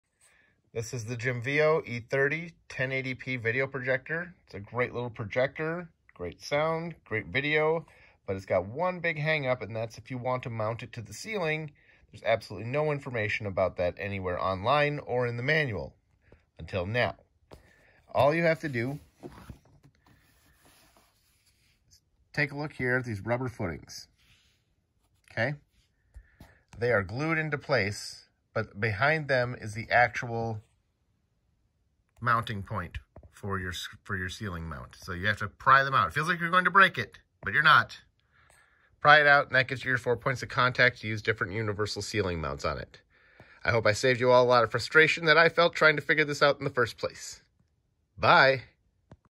Jimveo E30 Projector Ceiling Mounts
Here is where they have hidden your ceiling mounts on the Jimveo E30 Projector. You have to pry the glued down footers out to access them. This is nowhere in the manual and I couldn’t find it online.